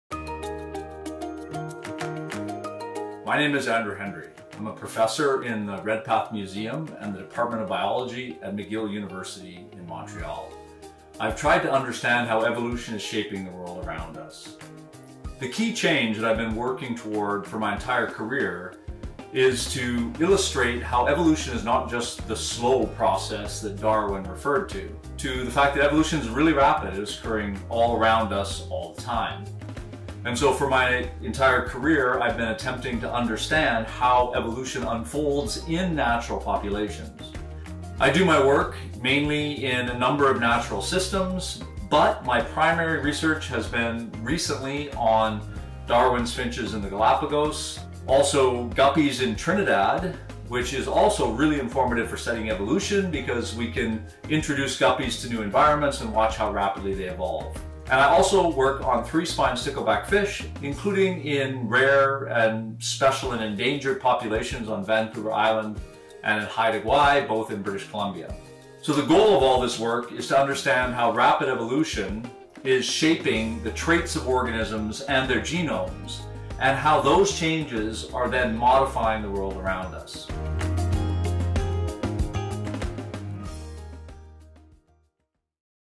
Researcher Spotlight - Andrew Hendry
Highlighting 2021 Royal Society of Canada Fellow, Andrew Hendry, Professor, Redpath Museum and Department of Biology.